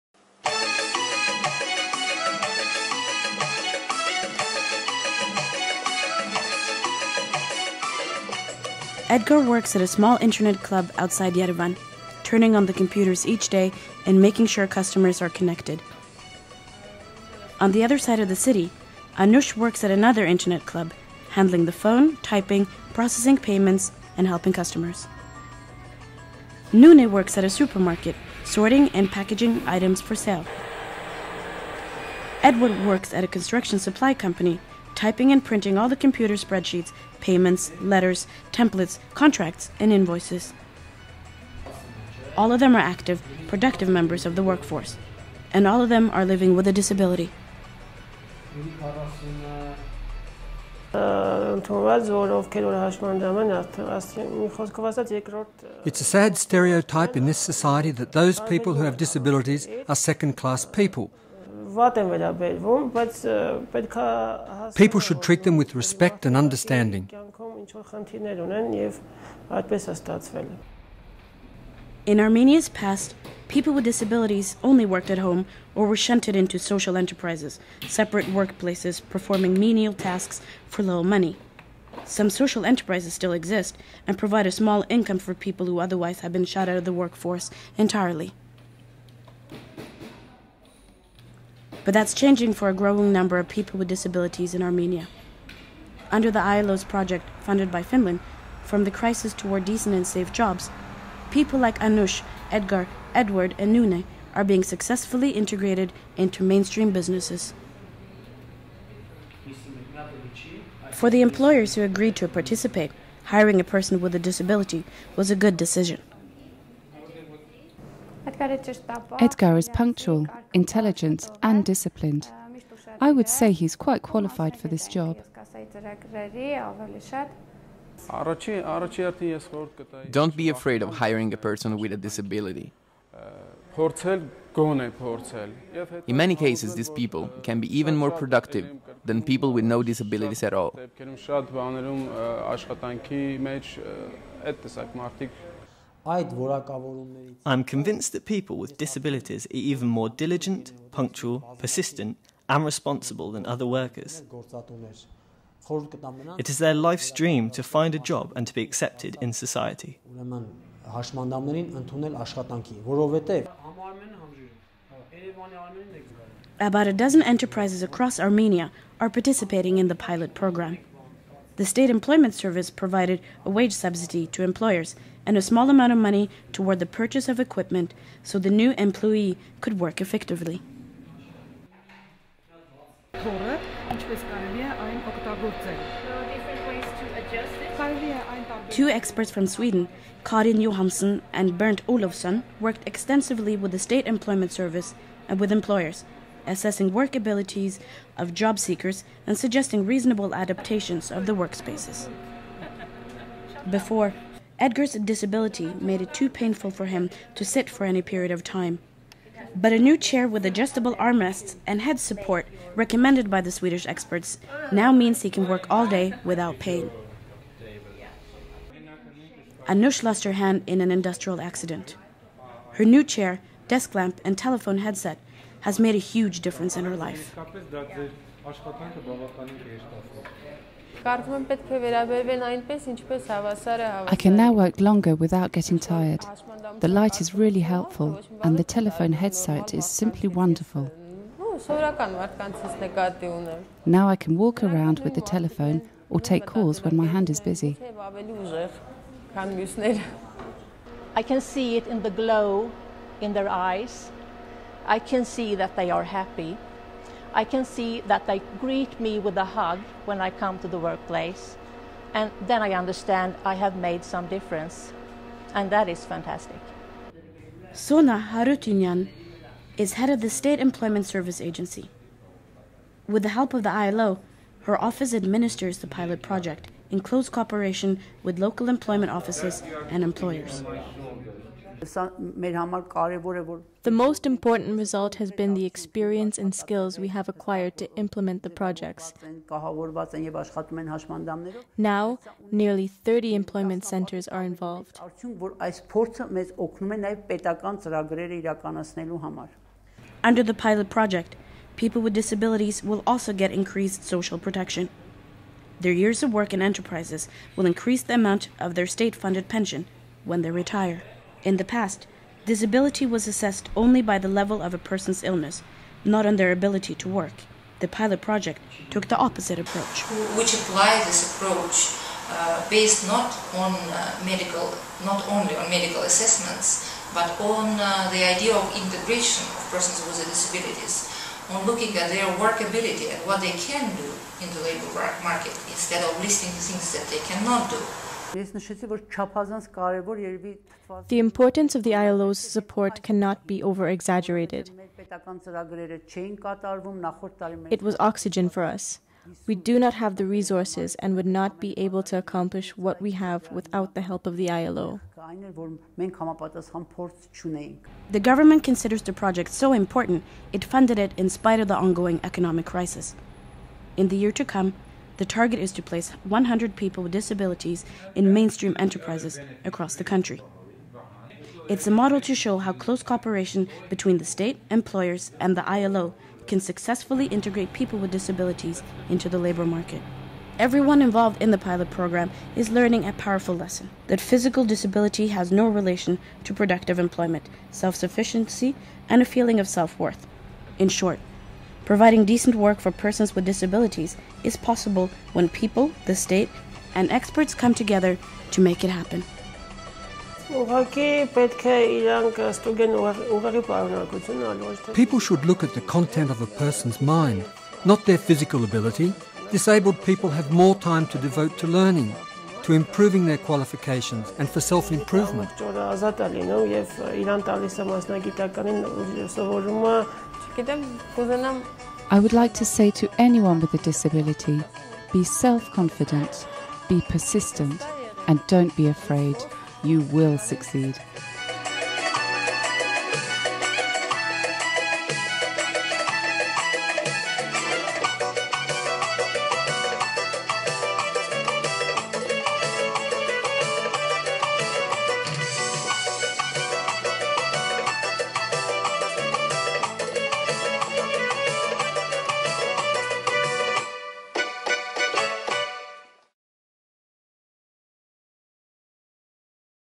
Breaking the barriers: people with disabilities in Armenia's labour market
Life in Armenia is changing, and for the better. After a disastrous earthquake and the end of Soviet communism, living standards for most people are steadily improving. But for people with disabilities, it's a different story. During the Soviet period, people with disabilities had some chances to work inside a closed system of "social enterprises." But now they have to compete with every other job-seeker in the labour market and are often shut out; they are truly "invisible" in the workforce. But the ILO's new pilot project in Armenia is changing that and at the same time, breaking down old stereotypes about disability and the barriers that keep people with disabilities out of the workforce. Written and produced for the United Nations/International Labour Organization.